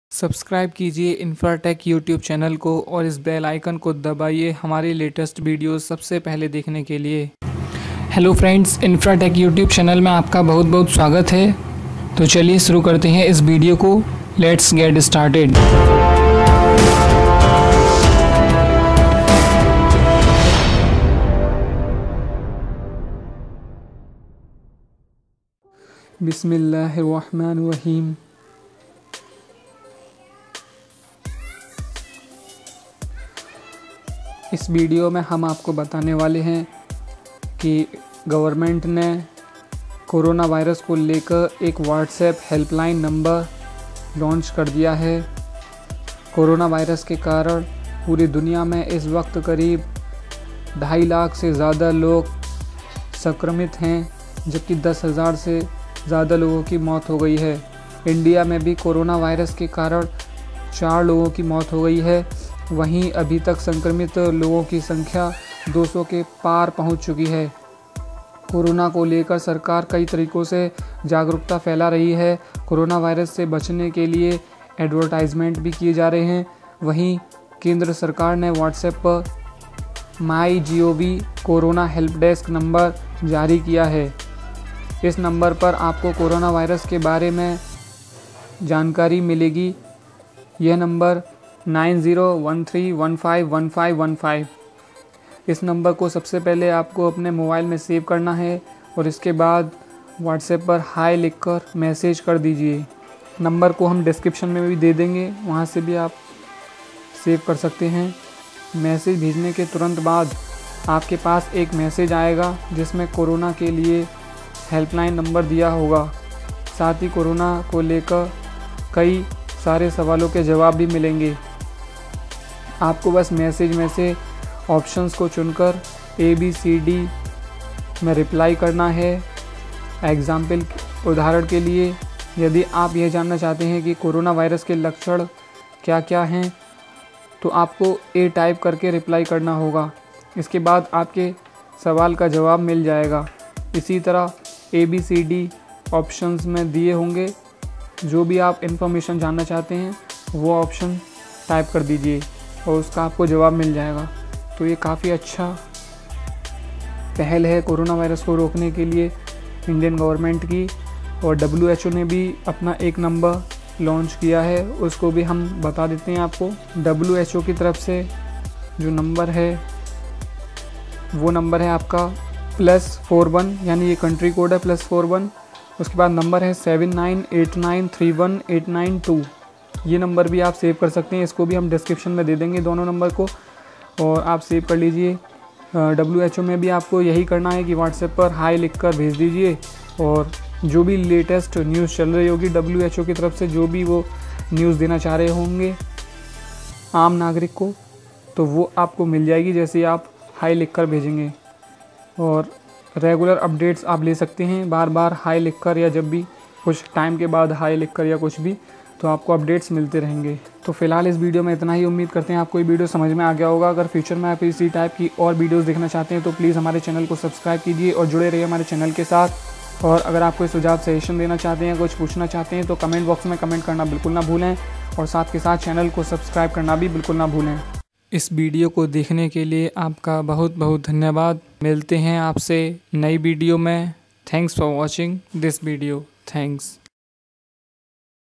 Indian Govt and WHO launch WhatsApp Helpdesk Number for latest updates of CORONA VIRUS
In this video I have told you about the Indian Govt and WHO launch WhatsApp Helpdesk Number for latest updates of CORONA VIRUS. Just Say Hiii. on WhatsApp & Get All Information regarding COVID-19.
THANKS

Indian Govt Number - (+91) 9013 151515
Save these number in your contact list & Say Hiii on WhatsApp